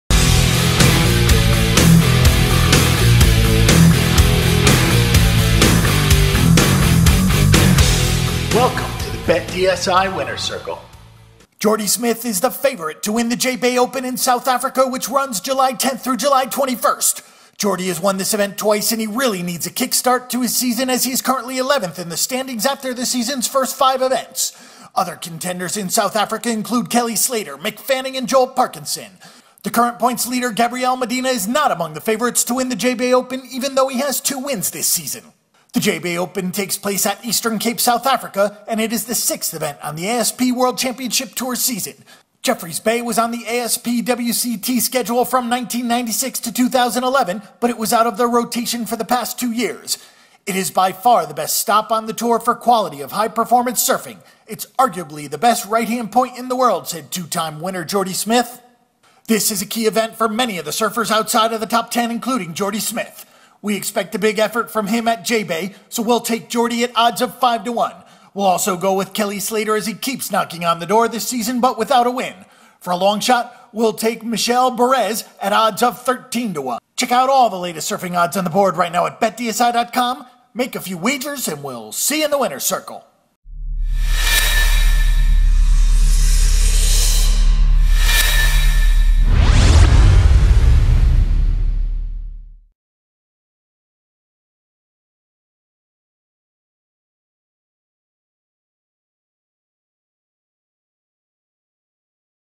J-Bay Open Odds and Surfing Betting Predictions
Jordy Smith is the favorite to win the J-Bay Open in South Africa which runs from July 10th through July 21st. Smith has won this event twice and he really needs a kick start to his season as he is currently 11th in the standings after five events. Other contenders in South Africa should include Kelly Slater, Mick Fanning and Joel Parkinson. The current point's leader, Gabriel Medina is not among the favorites to win the J-Bay Open even though he has two wins this season.

Here is a look at the J-Bay Open and surfing predictions.

J-Bay Open

The J-Bay Open takes place at Eastern Cape, South Africa from July 10th through the 21st. It is the sixth event on the ASP World Championship Tour season. Jeffreys Bay was on the ASP WCT schedule from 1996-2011 but it was out of the rotation the past two years. "It's by far the best stop on the tour for quality of high performance surfing. It's arguably the best righthand point in the world." said two-time winner Jordy Smith.

Top J-Bay Contender Odds

Jordy Smith 5-1

Kelly Slater 6-1

Mick Fanning 8-1

Joel Parkinson 8-1

Julian Wilson 10-1

Taj Burrow 10-1

Gabriel Medina 13-1

Adriano De Souza 12-1

John John Florence 13-1

Michel Bourez 13-1